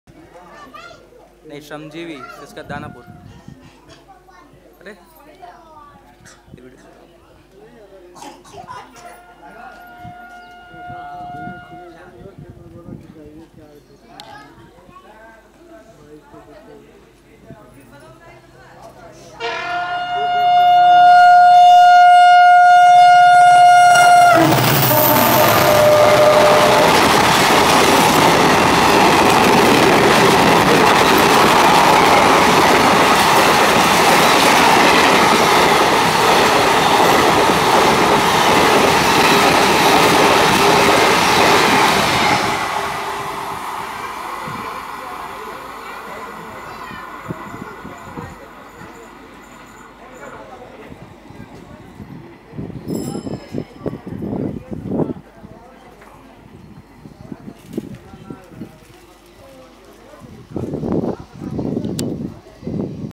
Crazily Honking EMD,WDP-4D With Patliputra-LTT Superfast Blasts Through Sakaldiha At 110Kmph
WDP-4D PATLIPUTRA-LT SF CRAZILY WHINNING AND BLASTS THROUGH SAKALDIHA AT 110KMPH ..